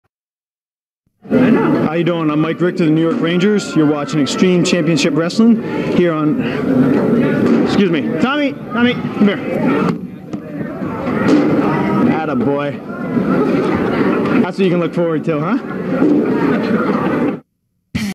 Nyisles interrupts Mike Richter during ECW promo (April 1996)
Nyisles, the short lived Islanders mascot appears at an Extreme Championship Wrestling taping with Ranger goalie Mike Richter and gets punched out by Tommy Dreamer